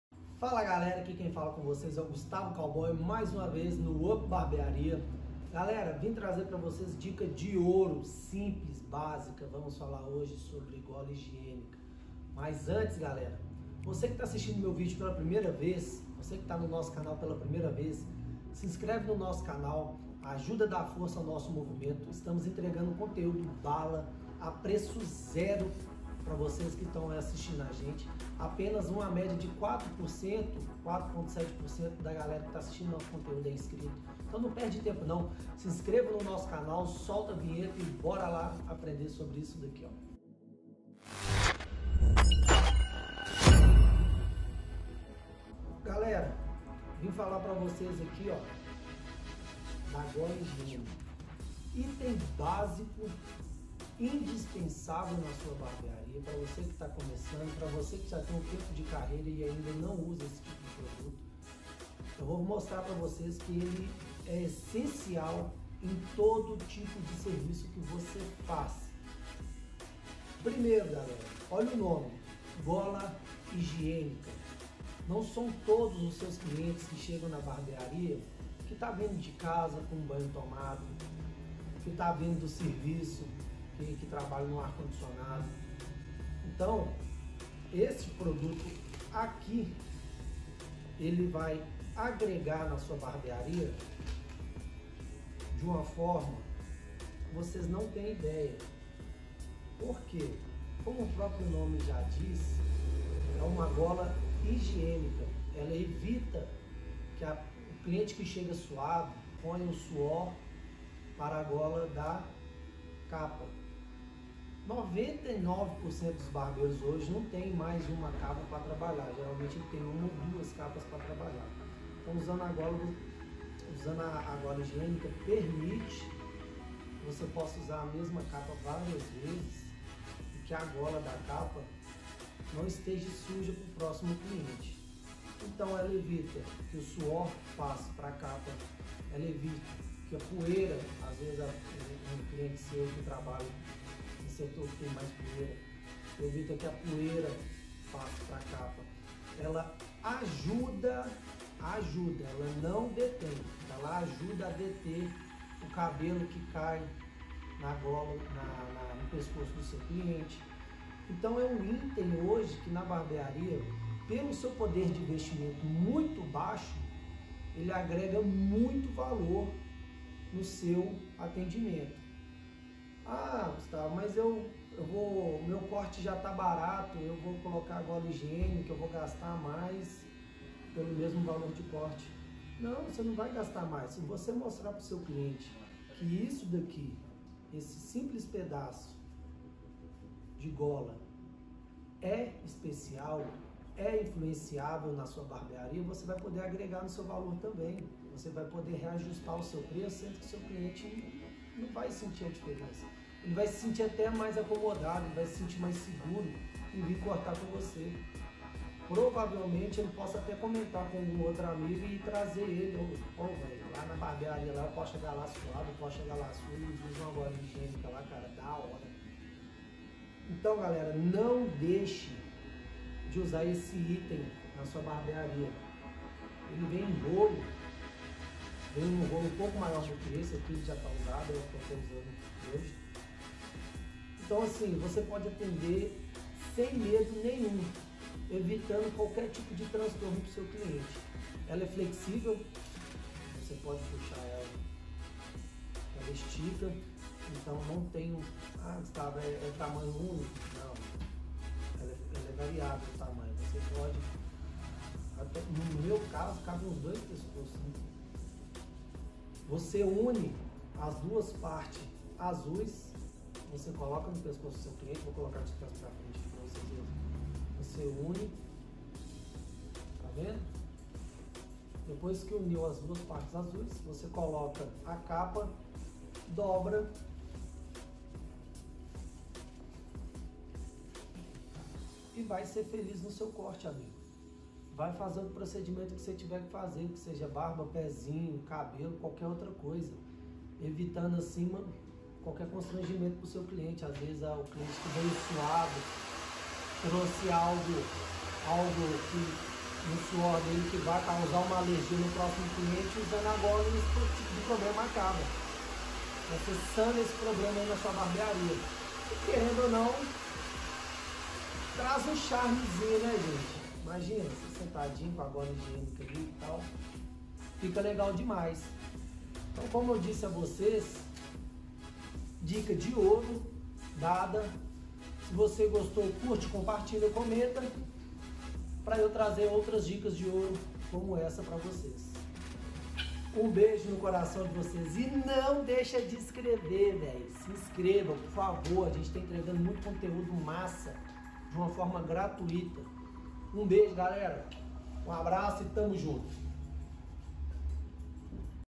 COMO USAR GOLA HIGIÊNICA BARBEARIA
Você sabe qual a verdade função da gola higiênica? Por qual motivo ela sempre deve ser usada? Fizemos um videos para tirar todas essas duvidas e ensinar você a usar a gola higiênica de forma correta!

Sobre o canal: UP BARBEARIA é um canal dedicada a elevar o nível da sua barbearia e contribuir com o seu desenvolvimento pessoal!